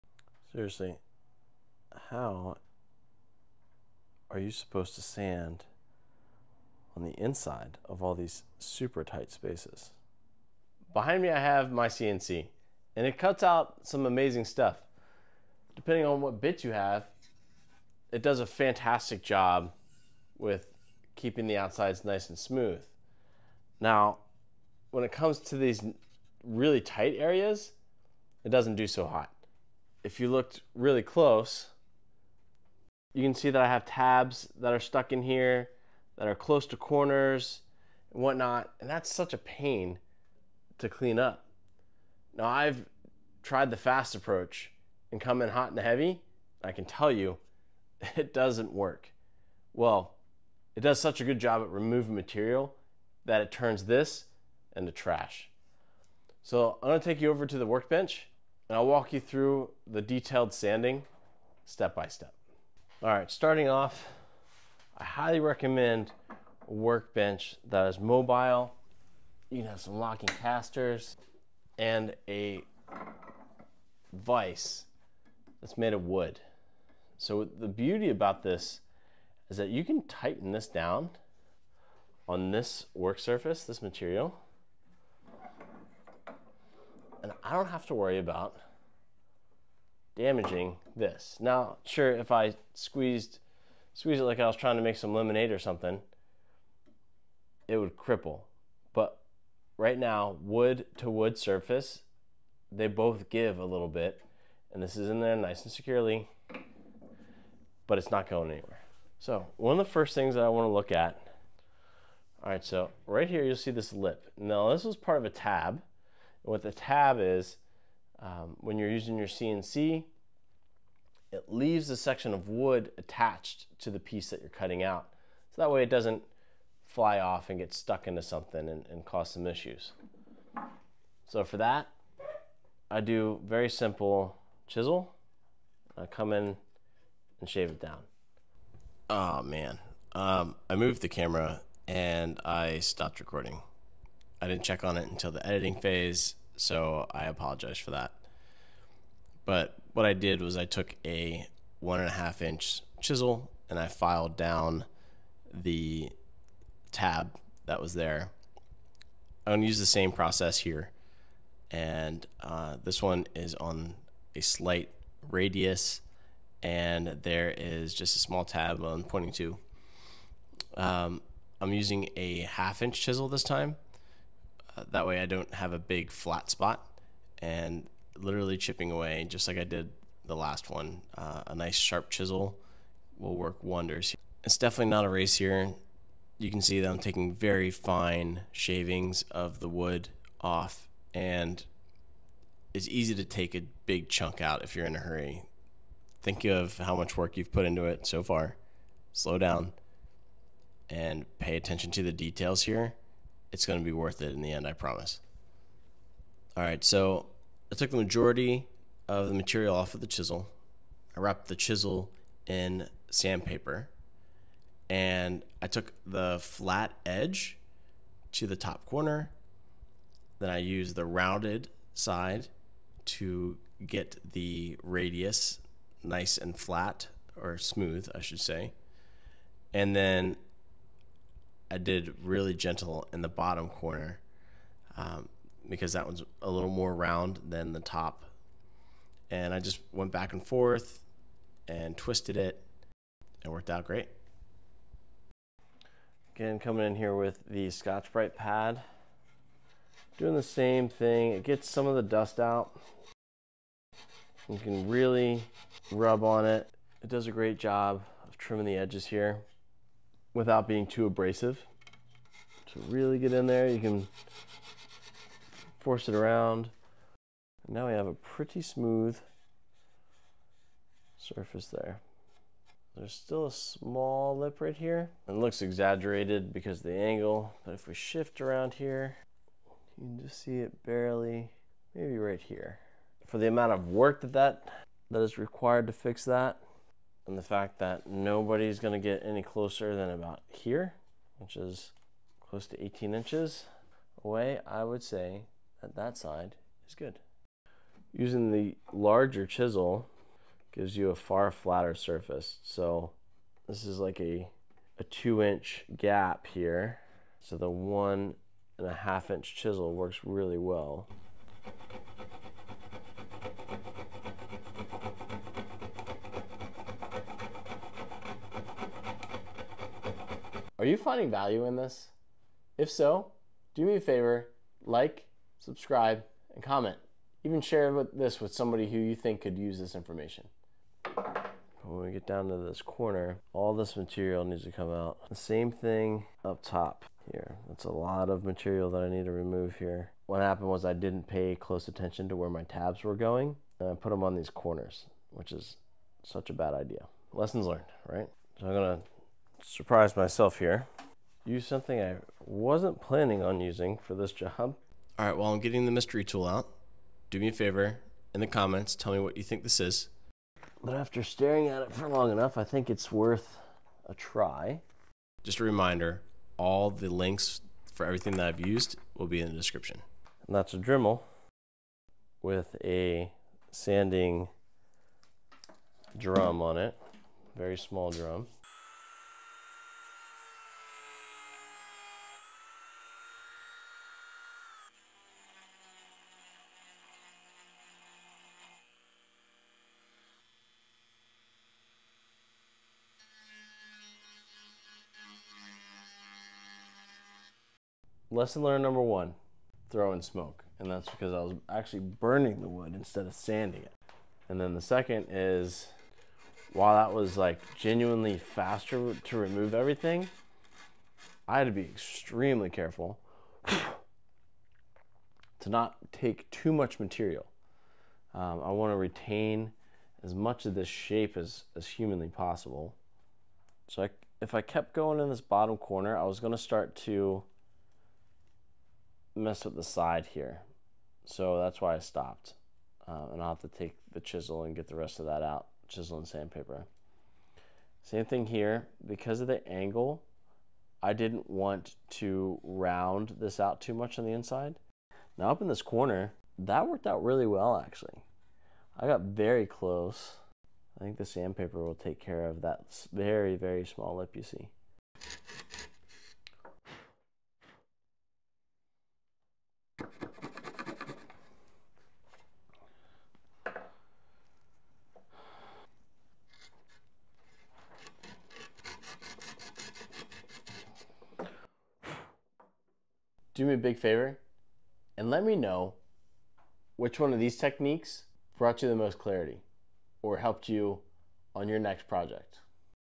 Unlocking the Art of Detail Sanding for Woodworking Newbies
Today we're diving deep into the world of detail sanding, specifically focusing on the intricate task of sanding the interior radius surfaces (using a CNC carved plaque).  Links to all products used is under the Chapter section.

Detail sanding is a critical step in woodworking that can make a significant difference in the overall quality and appearance of your finished project. Mastering this skill will not only enhance the look of your projects but also increase their durability and appeal. This video is perfect for beginners who are just starting their woodworking journey, as well as intermediate woodworkers looking to improve their sanding techniques. If you’ve ever struggled with getting a smooth finish on your intricate  projects, this tutorial is for you!

We'll demonstrate various sanding techniques to ensure you achieve a consistent and smooth surface. This includes tips on how to avoid common mistakes like over-sanding or creating uneven surfaces.

1selling Chisels on Amazon

3M Mesh sanding discs

Scotch Brite Pads

📌 Chapters:
00:00 Before sanding overview
01:02 Workbench recommendation
01:50 What is a "tab"
02:15 How to remove the left over tab
02:50 Using different chisels for bulk material removal
03:30 Using the chisel as a sanding block
04:05 Using a Scotch Brite Pad
05:15 Chisel AMSR
05:35 Bulk Material Removal
07:55 Final Product